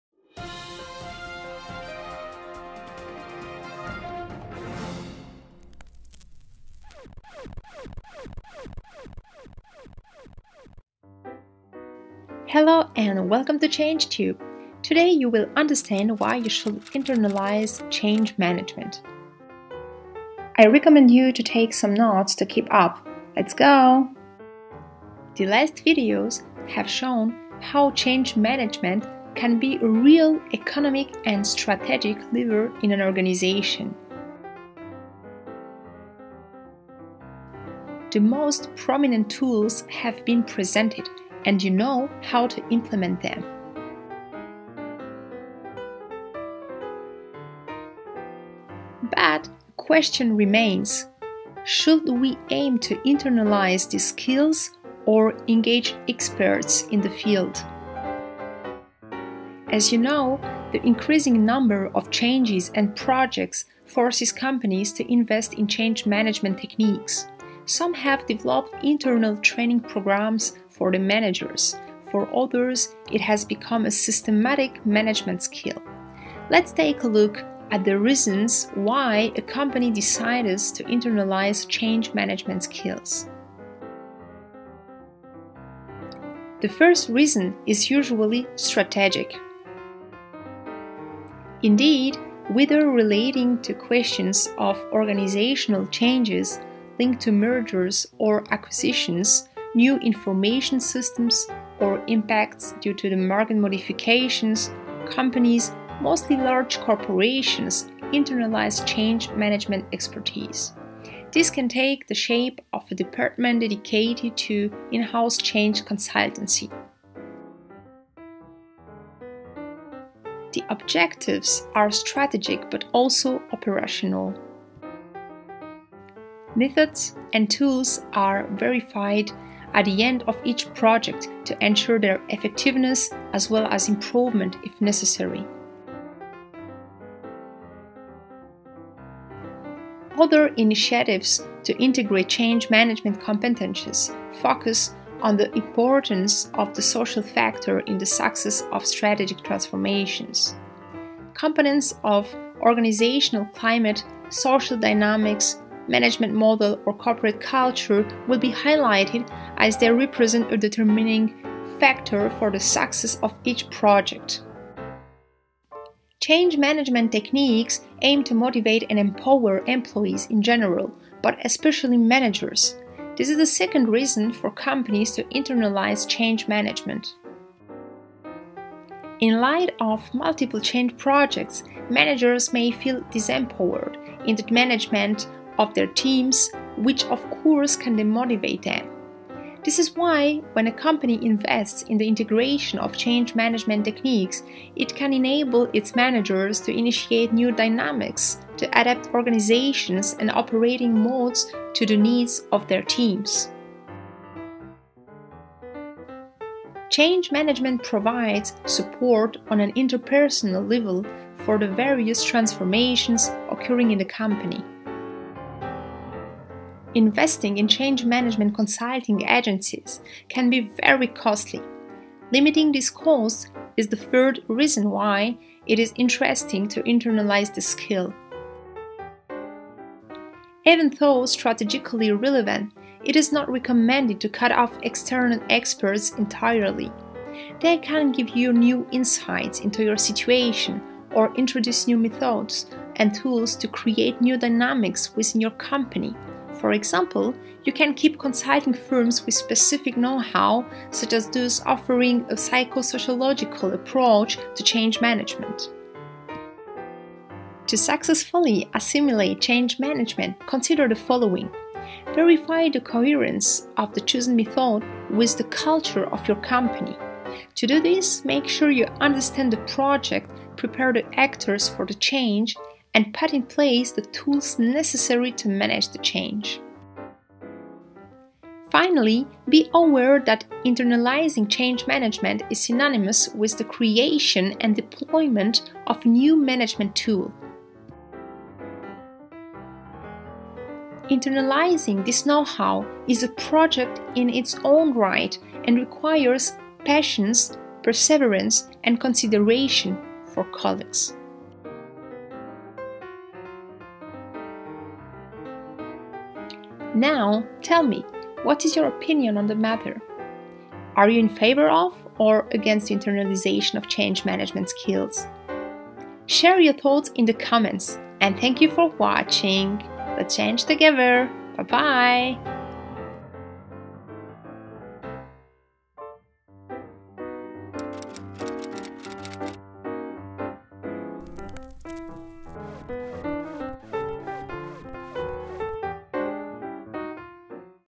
CHANGE MANAGEMENT | Step #8 | Internalizing Change
In this video you'll understand why you should internalize change management skills. Have fun and keep on watching! 📺🤓